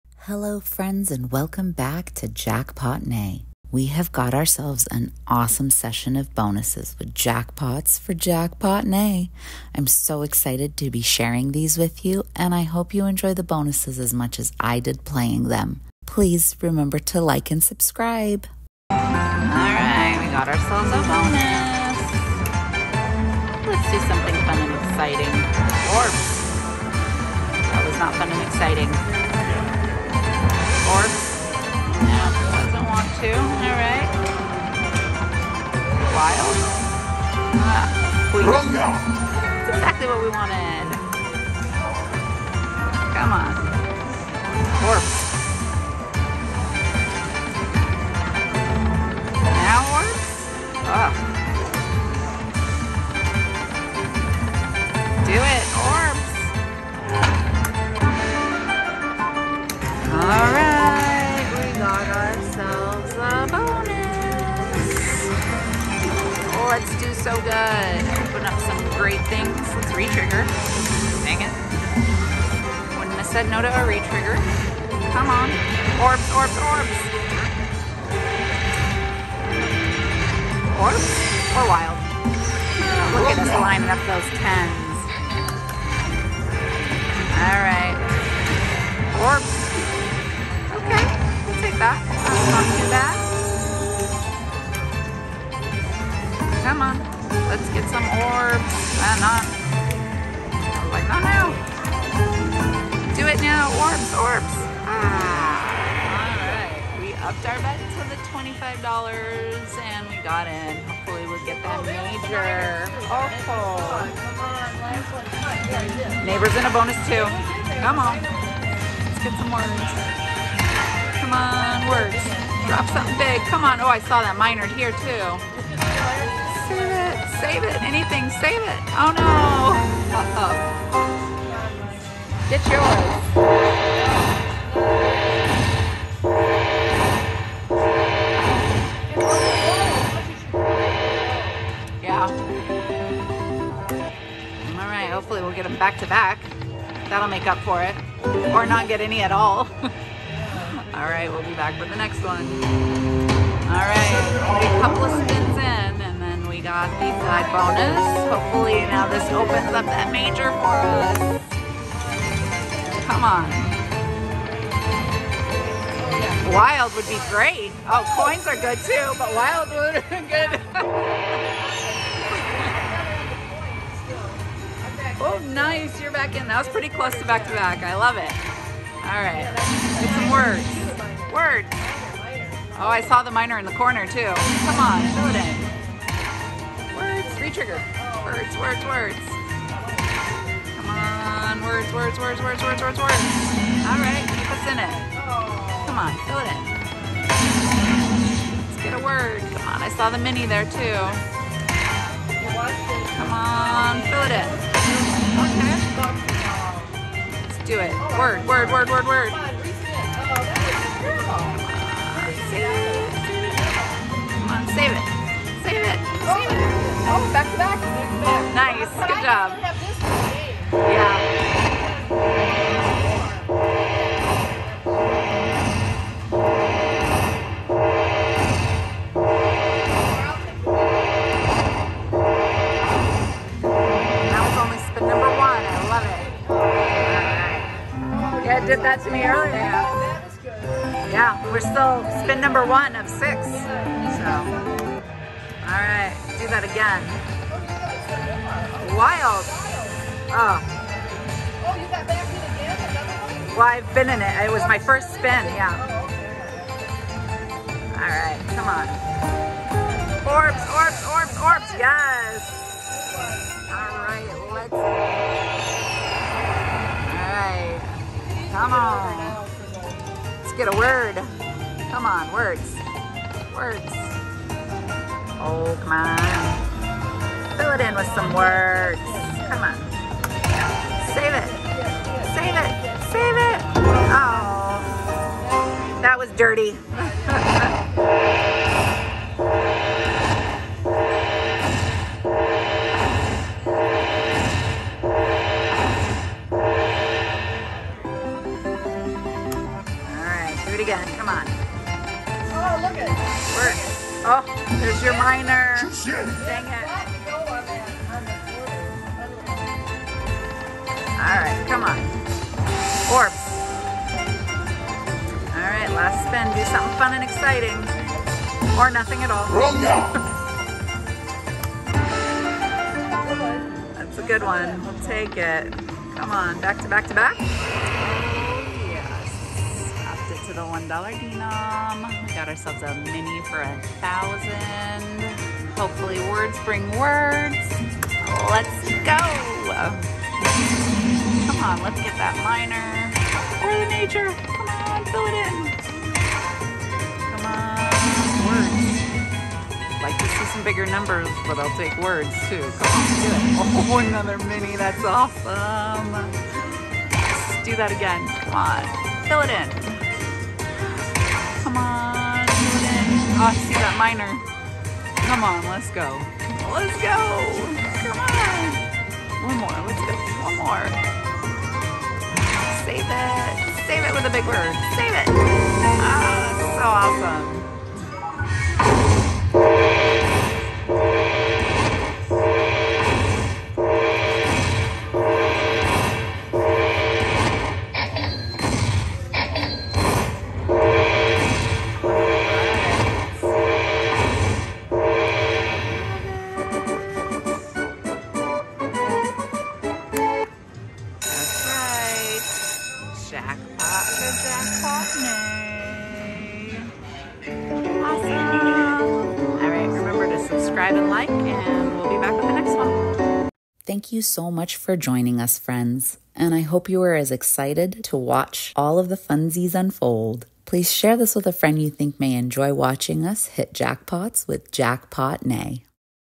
Dragon Cash REALITIES with a FINAL JackPot for JackPot Nay!
Hey friends! I'm excited to show you these bonuses and JackPot we got on Dragon Cash!